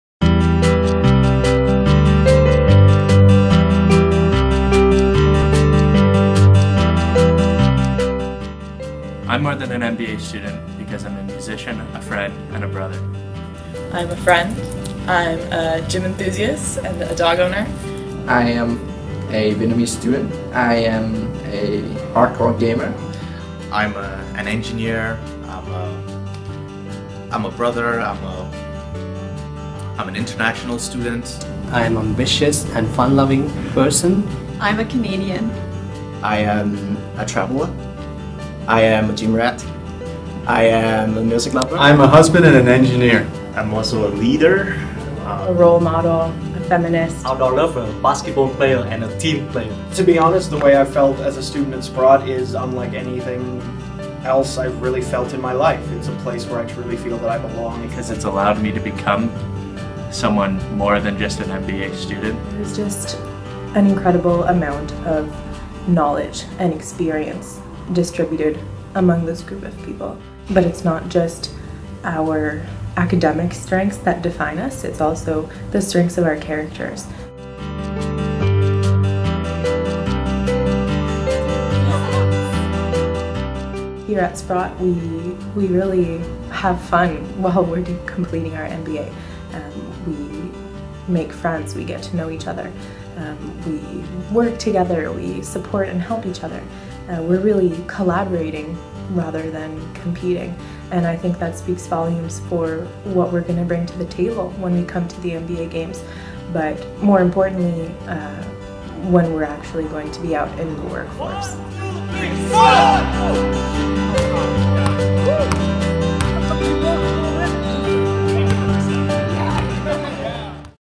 Sprott MBA Games 2016
In January 2016, the Sprott School of Business will send 27 MBA students to the annual MBA Games competition. Get to know the team and discover how they are much more than just MBA students.

For more information about the Sprott School of Business, please